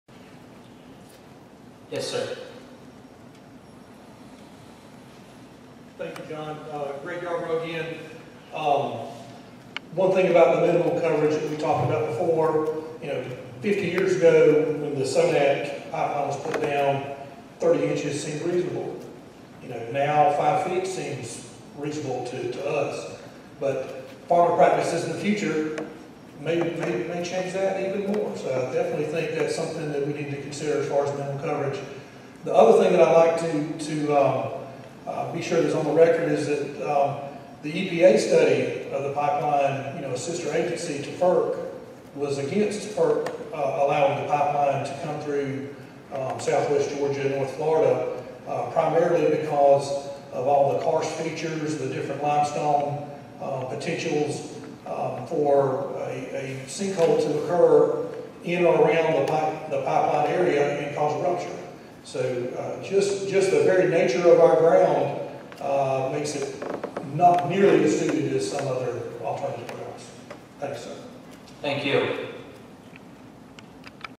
5 feet top cover probably not even enough; EPA abt karst --Greg Yarborough @ FERC 2015-09-29
FERC in Moultrie, GA without Sabal Trail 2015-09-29